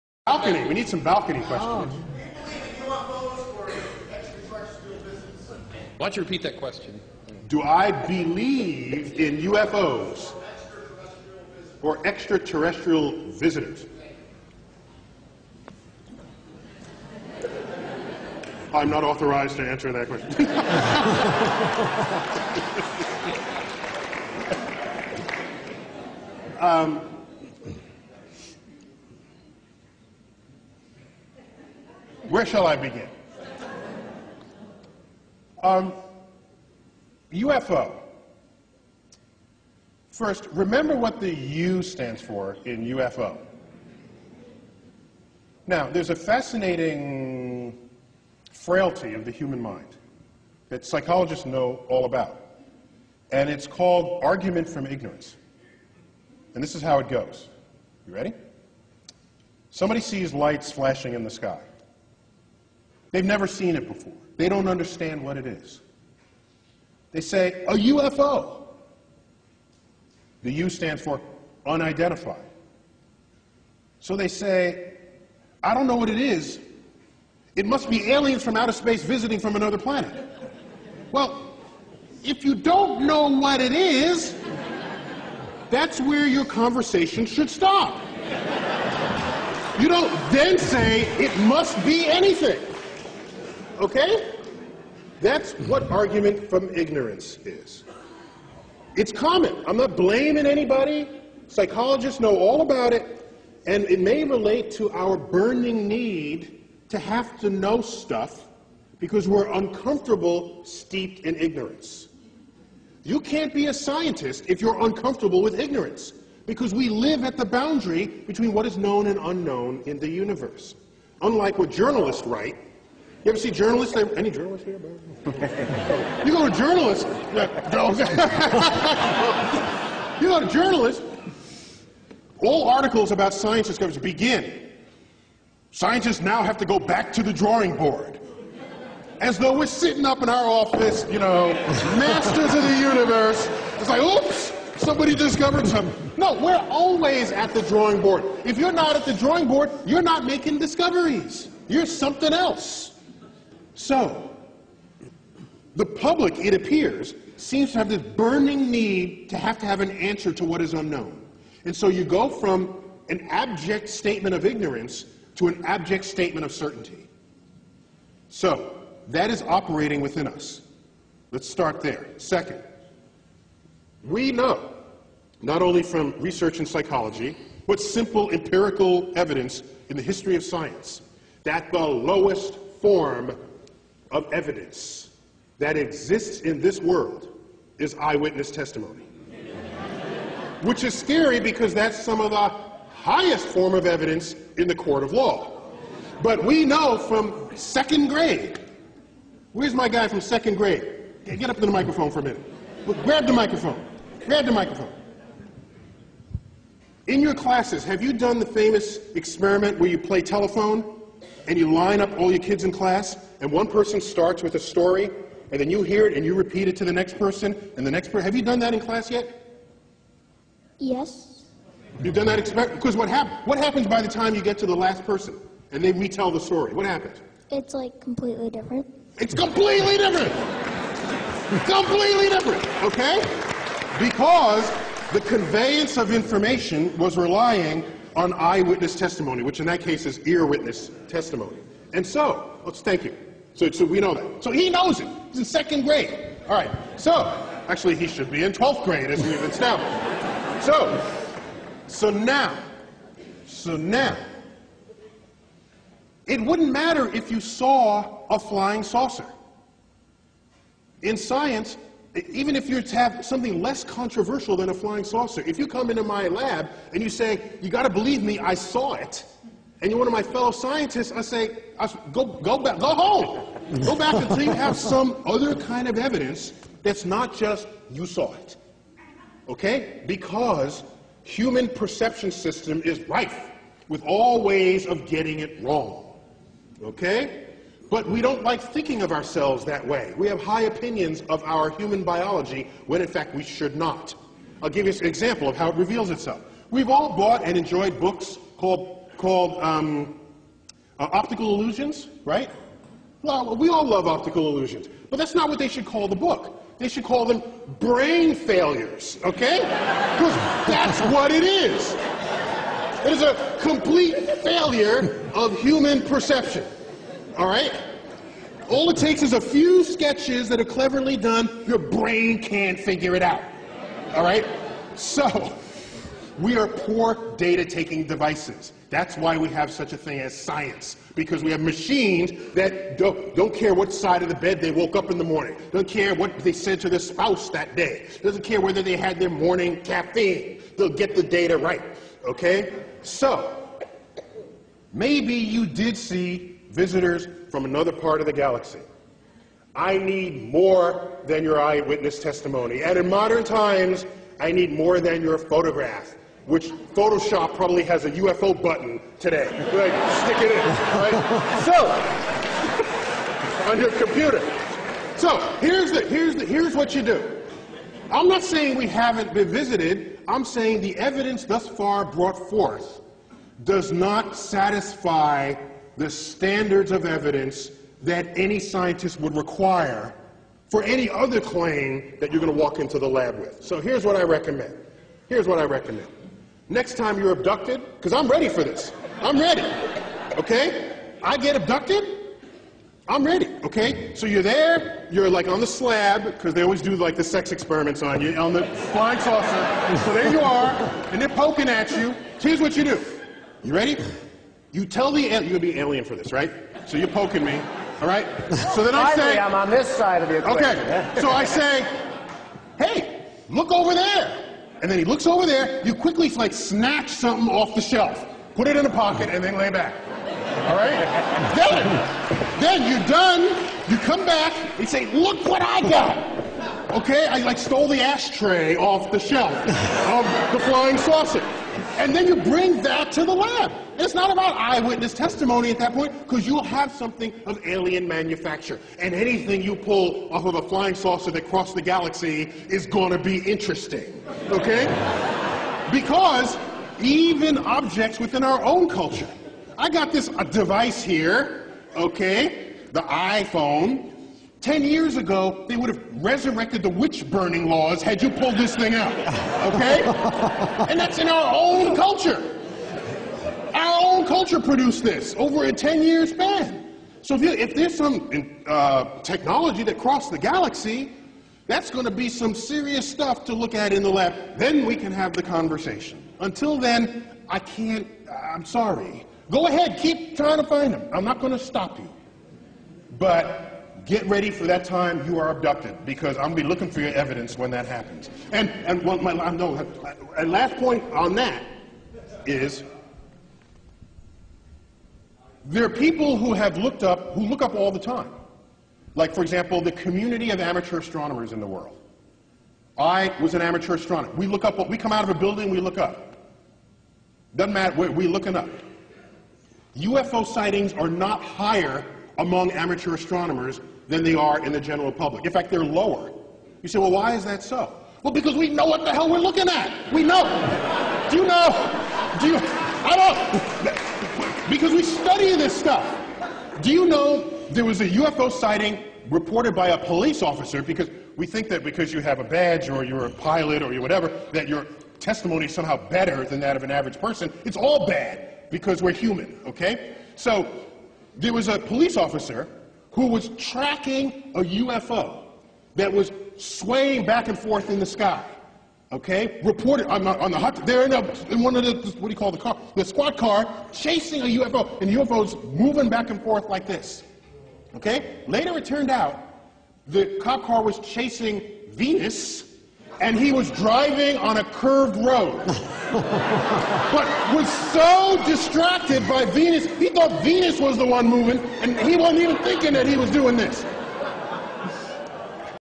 Neil deGrasse Tyson hablando sobre OVNIS
Neil deGrasse Tyson es un astrofísico, escéptico y divulgador científico estadounidense además de director del Planetario Hayden del Museo Nacional de Historia Natural de Nueva York.

Es cercano y agradable. Siempre que tiene una oportunidad habla de ciencia con humor. Y muchos damos fe de que lo consigue. Es sin duda, uno de los grandes divulgadores científicos de hoy en día.

En el siguiente video, a Neil deGrasse Tyson le preguntan sobre los OVNIS. En su respuesta se declara escéptico de las abducciones y platillos volantes. Además explica falacias lógicas como el argumento de la ignorancia o uno de mis preferidos, el argumento de la autoridad. Esta falacia suele ser muy utilizada en el mundo de la ufología donde a menudo se nos muestran testimonios que provienen de pilotos, militares, etc. cuando estos tienen tanta validez como el testimonio de un oso panda. Lo que necesitamos son pruebas. Pruebas palpables. Como decía Sagan, afirmaciones extraordinarias requieren pruebas extraordinarias.